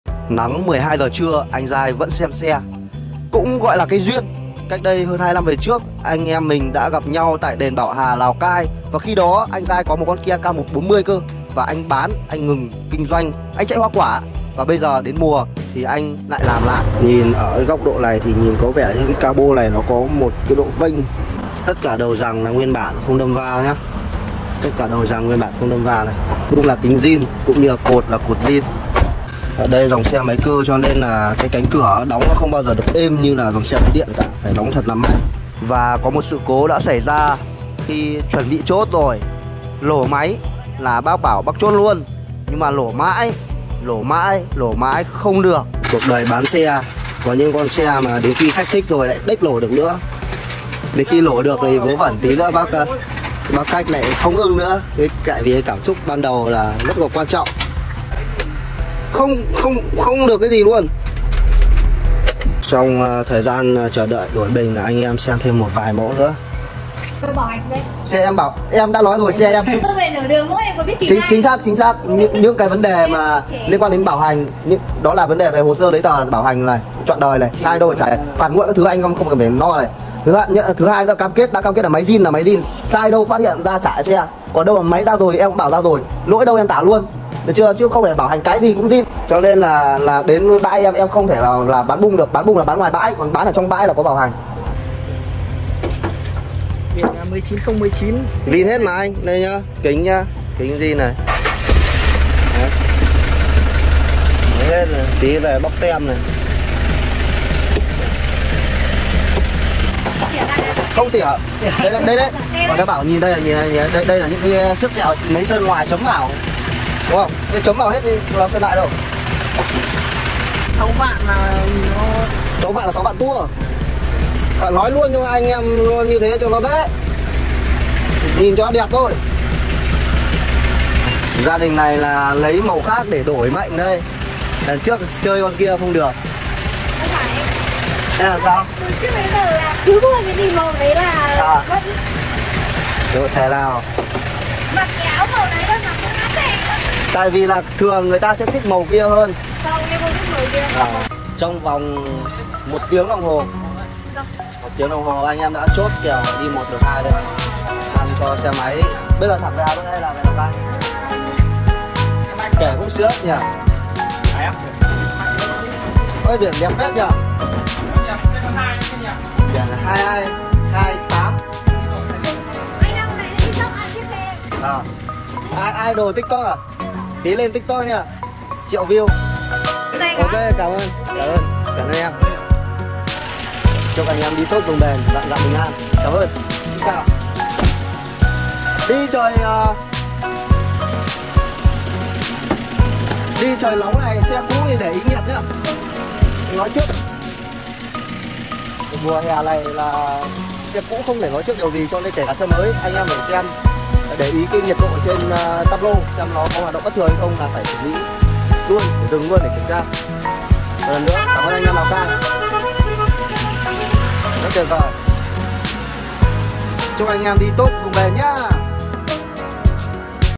Nói dối thì MẤT DẬY, mà Nói thật thì MẤT KHÁCH - Cuộc phiêu lưu mua xe của a zai Lào Cai
em ít oline,  anh thiện chí alo e hoặc nhắn tin e gọi lại, nhắn tin zalo e cũng được e để a giá tốt nhất ạ: ĐT/Zalo: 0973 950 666